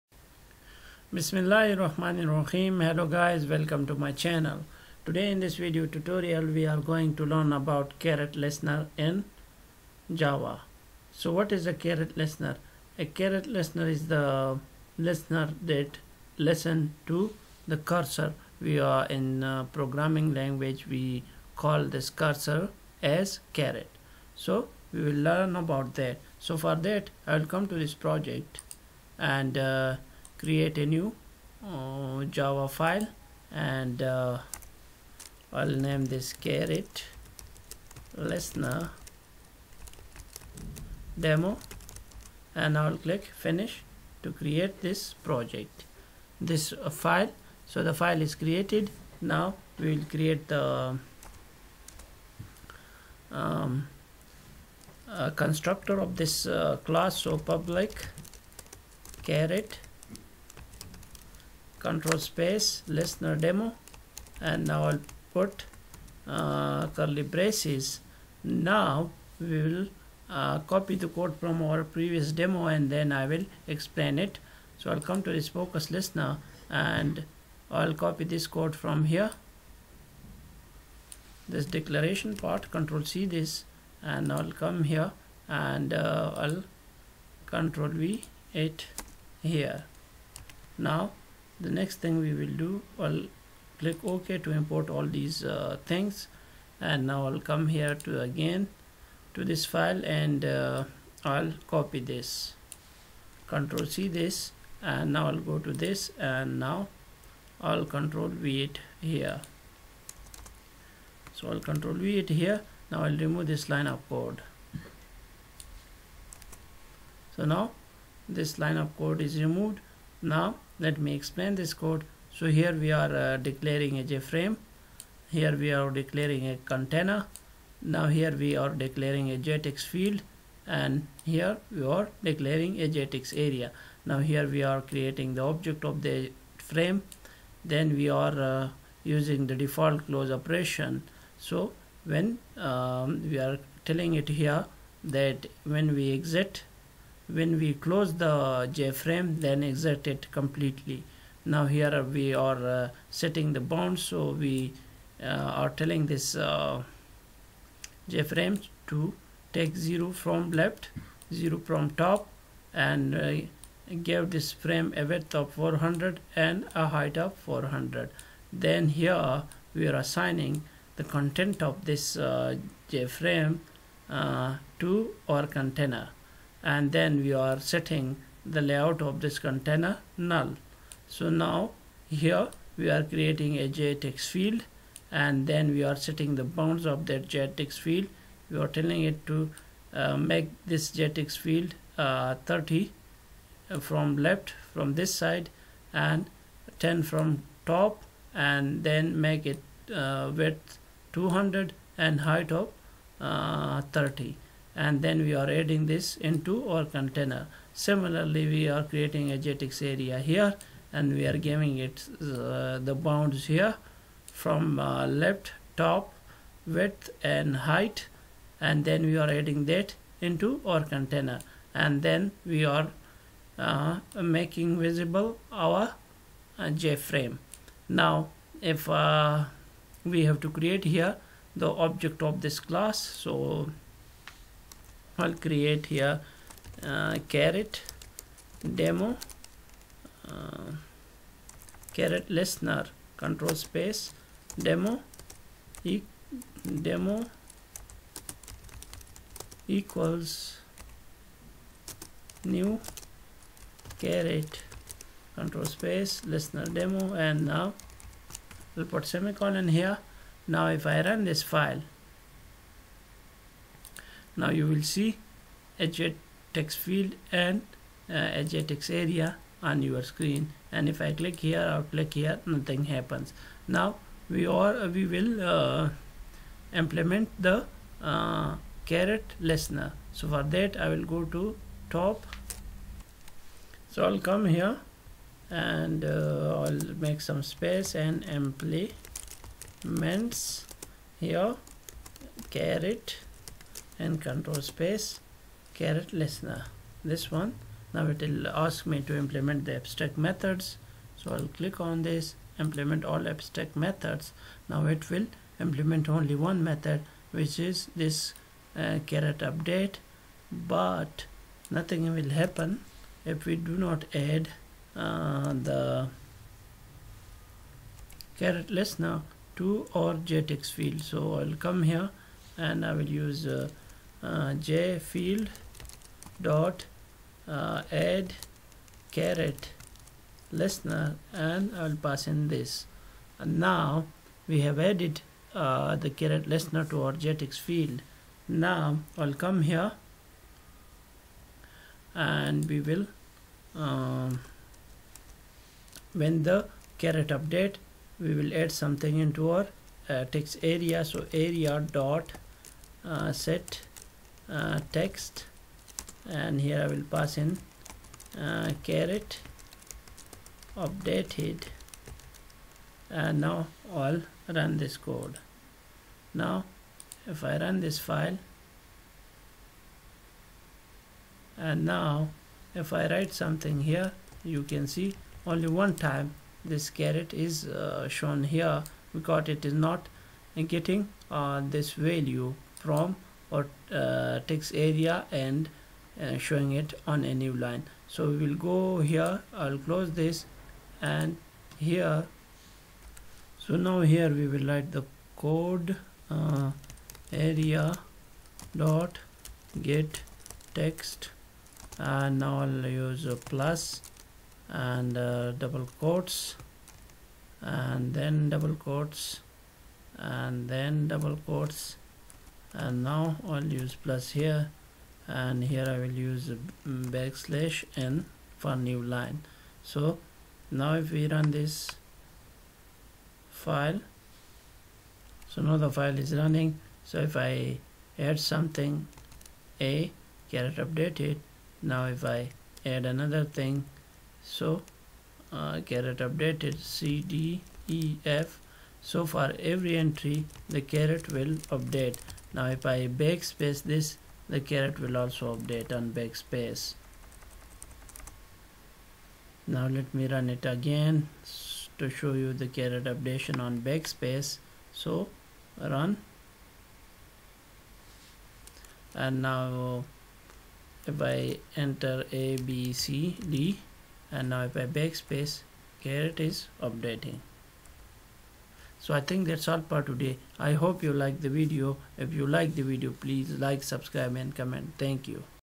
Java Programming | CaretListener java | Java Caret Listener | Java Swing Tutorial | Java GUI
How to Write a Caret Listener

Caret events occur when the caret — the cursor indicating the insertion point — in a text component moves or when the selection in a text component changes. The text component's document can initiate caret events when it inserts or removes text, for example. You can attach a caret listener to an instance of any JTextComponent subclass with the addCaretListener method.
Note: An alternate way of detecting caret changes is to attach a listener directly to the caret object itself rather than to the text component that manages the caret. A caret fires change events (not caret events), so you would need to write a change listener rather than a caret listener.

Caret Events and Listeners : CaretListener « Swing Event « Java Tutorial

Whenever a caret changes its position or if text is selected, a caret event is fired by a text component. The class CaretEvent supports the methods getDot() and getMark() to retrieve the current location and the end position of a text selection, respectively.
Java Code Examples for javax.swing.event.CaretListener
The following examples show how to use javax.swing.event.CaretListener. These examples are extracted from open source projects. You can vote up the ones you like or vote down the ones you don't like, and go to the original project or source file by following the links above each example. You may check out the related API usage on the sidebar.